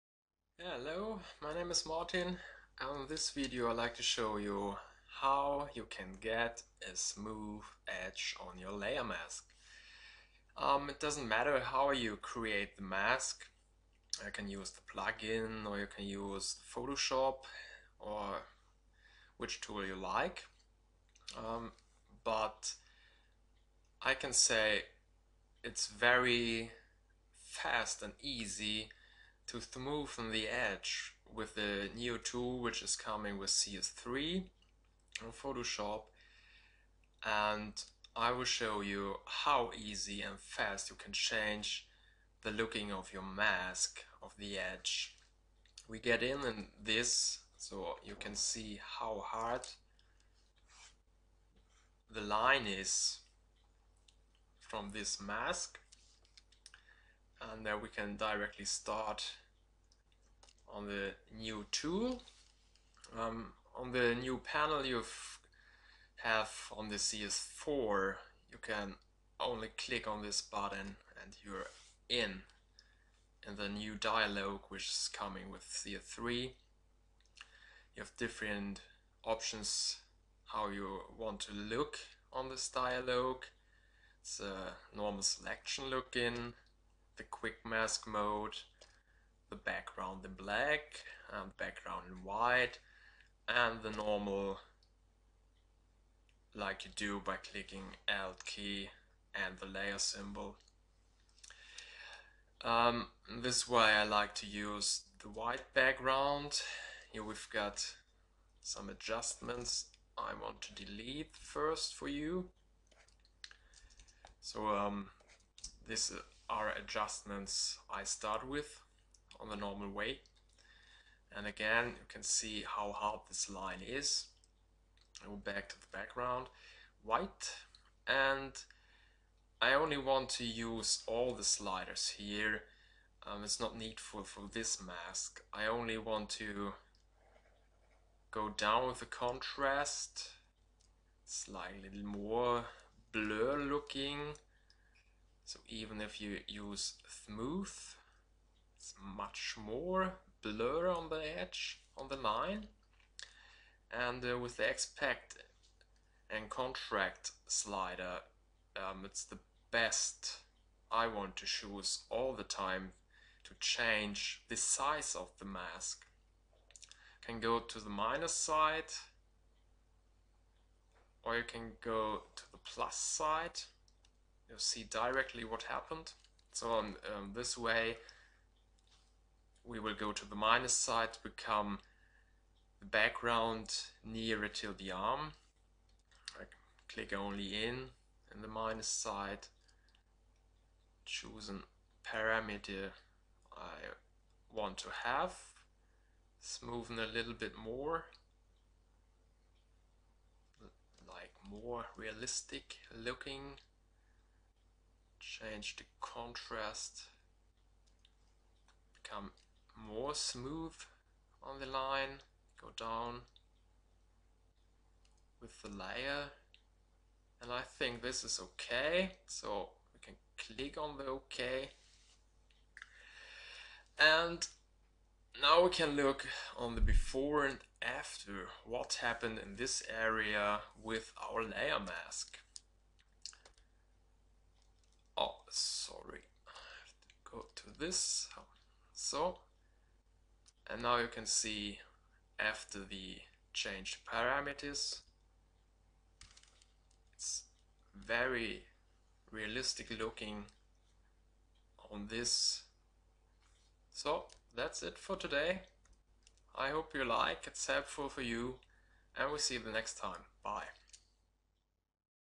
Mask with a smooth edge
This is a video showing how to become a smooth edge on a layer-mask.
It doesn't matter how you create your mask, which toll or plug-in you use. But to become a smooth edge I like to use the tool which is first comming with CS3.